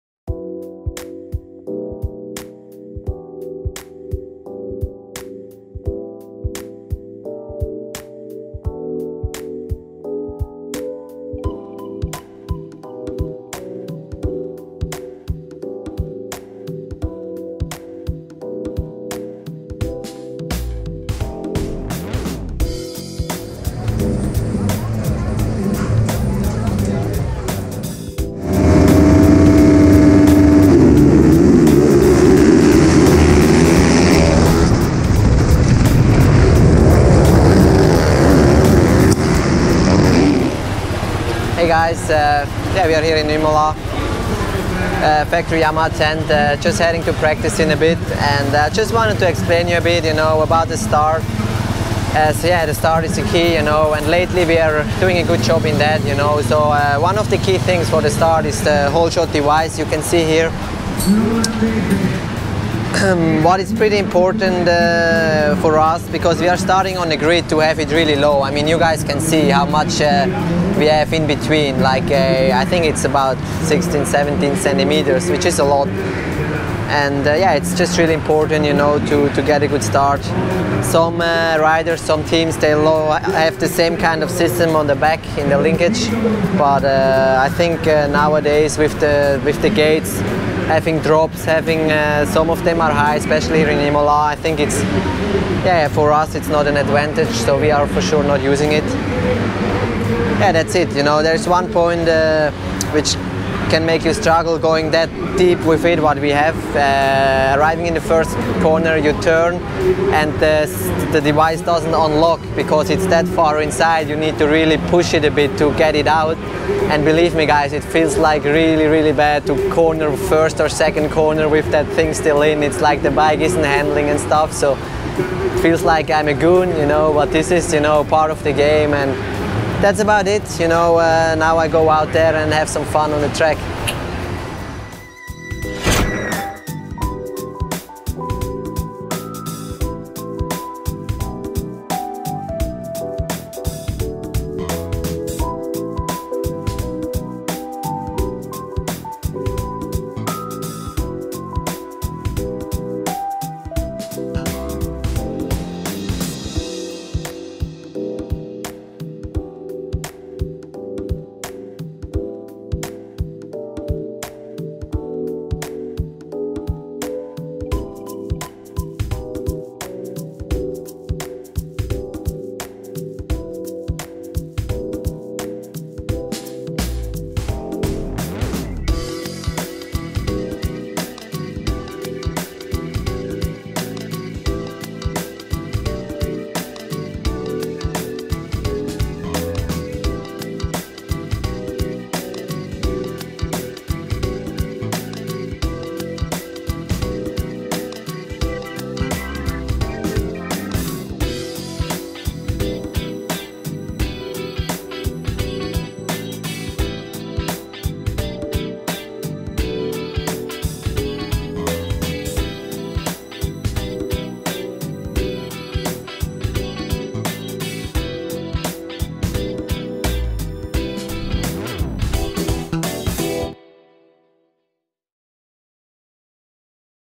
Jeremy Seewer Explains Yamaha Factory Start Device
🇮🇹 What type of start device does the Monster Yamaha Factory MXGP team use on their YZ450FM's?
With the start being so important in MXGP, we thought we’d talk to one of our best starters,Jeremy Seewer #91 about his start device.